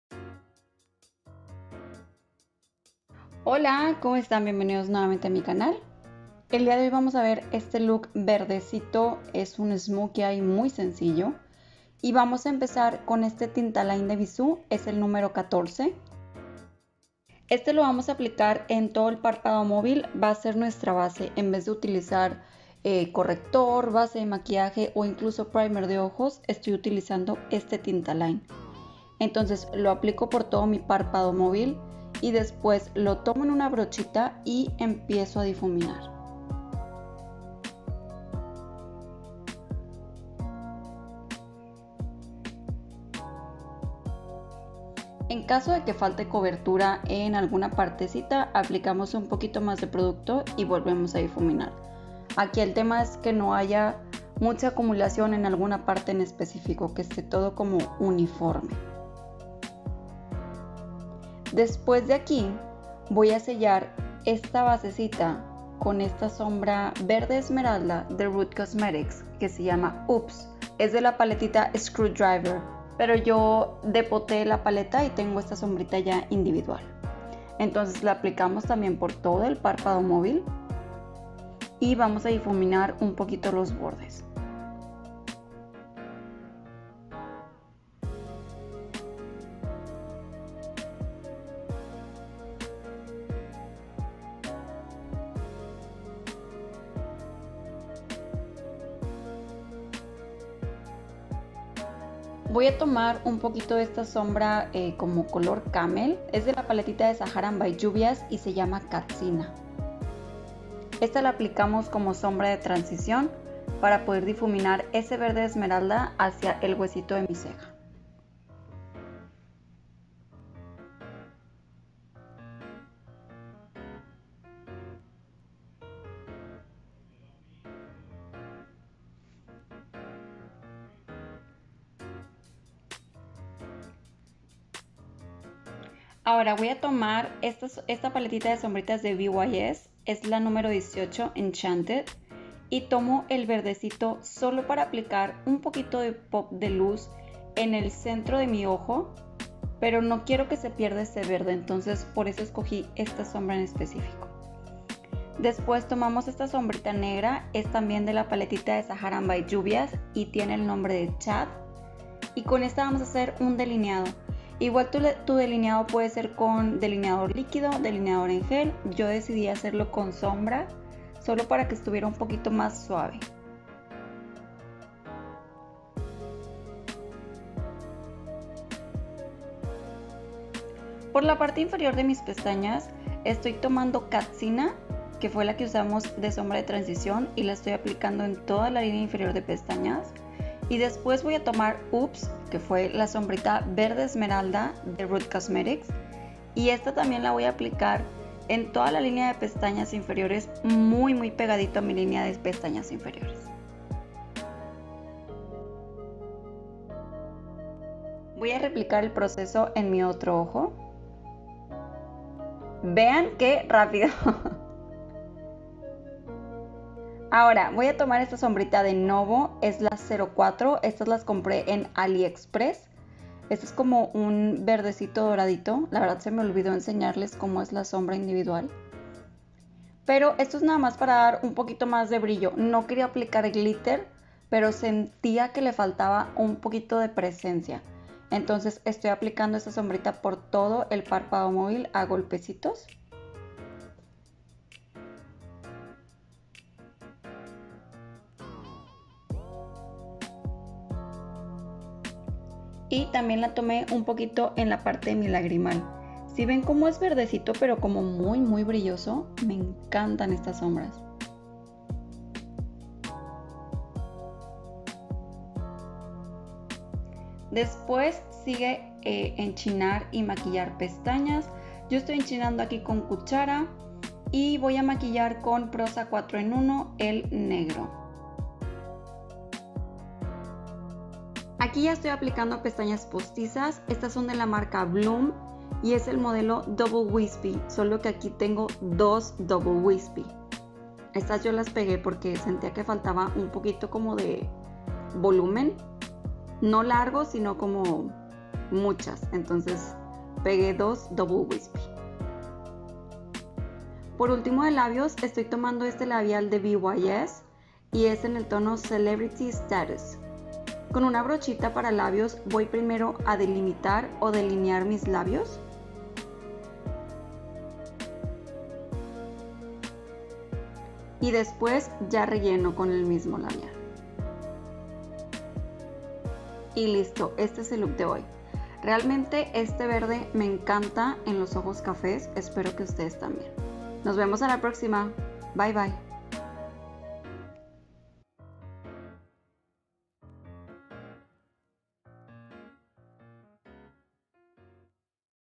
Smokey Eyes verde esmeralda | Liz Guajardo MUA
El tutorial del hoy es mega sencillo y con un color que me encanta ultimamente. Espero lo disfruten

- Productos utilizados - 🤓
Sombras
Rude Cosmetics Screwdriver - Oops!
Juvia's place The Saharan - Katsina y Chad
BYS Cosmetics Enchantes #18
Novo #04

Bissú Cosmetics Tintaline #14

BYS Matte Lipstick - Celebrity Status